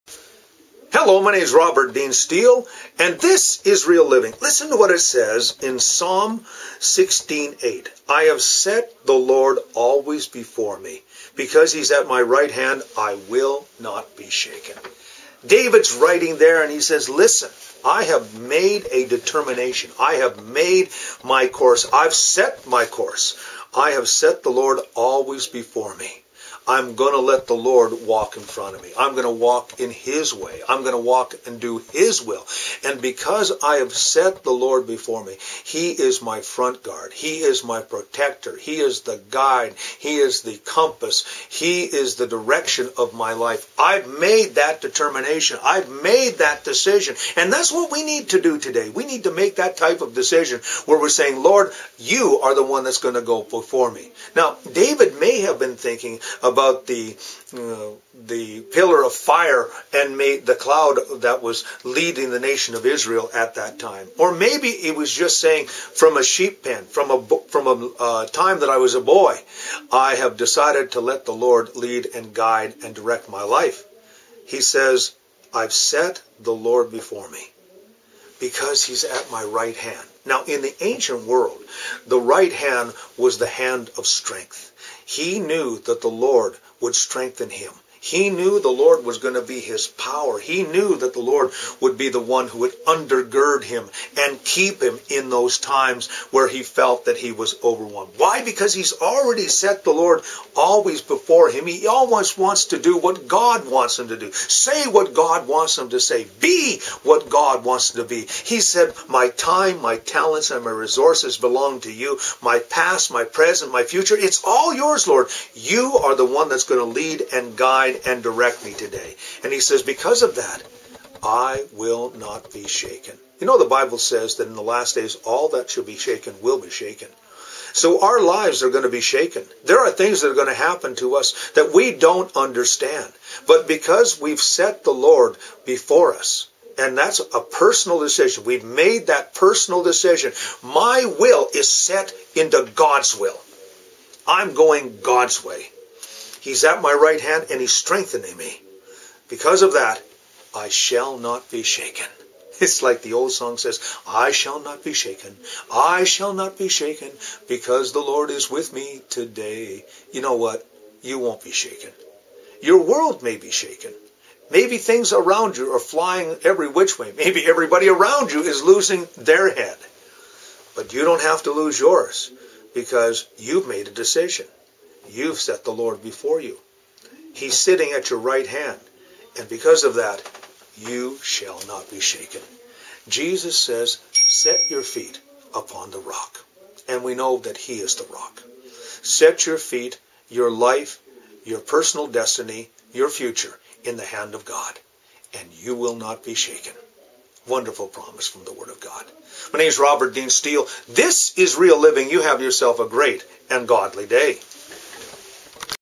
Set the Lord before you
Robert Dean Steel shares about how we set the Lord before us.